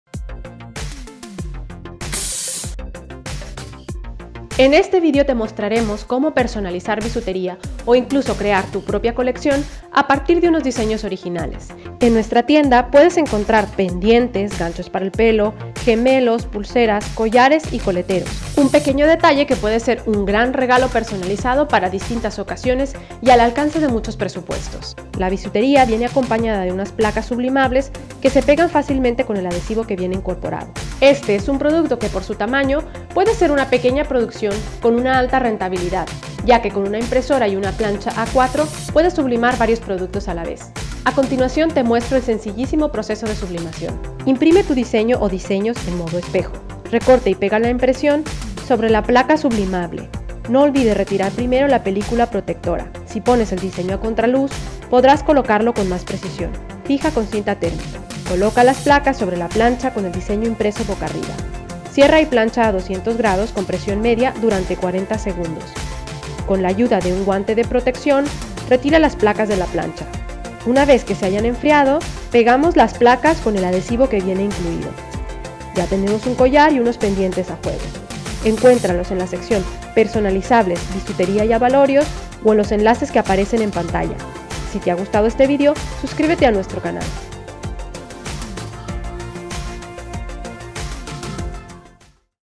Personalizar bisutería por sublimación
Ahora puedes personalizar bisutería o incluso crear colecciones a partir de tus propios diseños.

En este vídeo te mostramos el proceso de sublimación:

•Imprime tu diseño en modo espejo.
•Recorta y pega la impresión sobre la placa sublimable.*no olvides quitar primero la película protectora.
•Fija con cinta térmica. (Si pones el diseño a contraluz podrás colocarlo con más precisión)
•Coloca las placas sobre la plancha con el diseño impreso boca arriba.
•Cierra y plancha a 200º con presión media durante 40 segundos.
•Con la ayuda de un guante de protección retira la placas de la plancha.
•Una vez se hayan enfriado, pega la placa con el adhesivo que viene incluido.

Encuéntralos en la sección “personalizables – bisutería y abalorios”